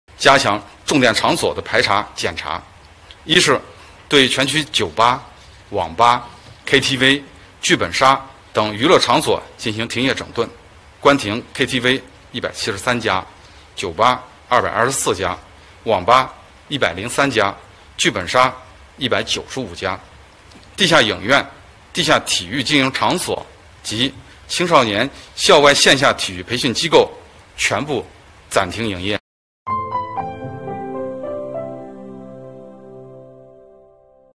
北京朝阳全区酒吧、KTV等娱乐场所停业整顿
6月14日，在北京市新型冠状病毒肺炎疫情防控工作新闻发布会上，朝阳区副区长孟锐介绍，朝阳区对全区酒吧、网吧、KTV、剧本杀等娱乐场所进行停业整顿。